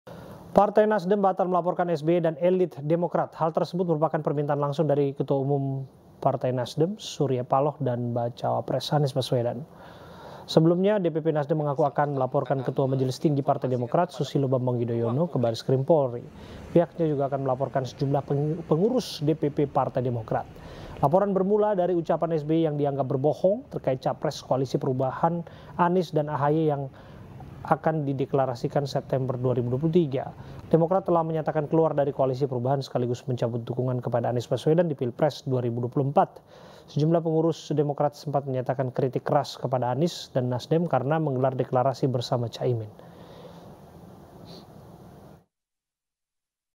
Nasdem Anggap SBY Keluarkan Pernyataan Bohong
Bendahara Umum Partai NasDem Ahmad Sahroni batal melaporkan Ketua Majelis Tinggi Partai Demokrat Susilo Bambang Yudhoyono (SBY) ke Badan Reserse Kriminal (Bareskrim) Polri terkait berita hoaks atau bohong. Pembatalan itu dilakukan karena larangan dari Ketua Umum NasDem Surya Paloh.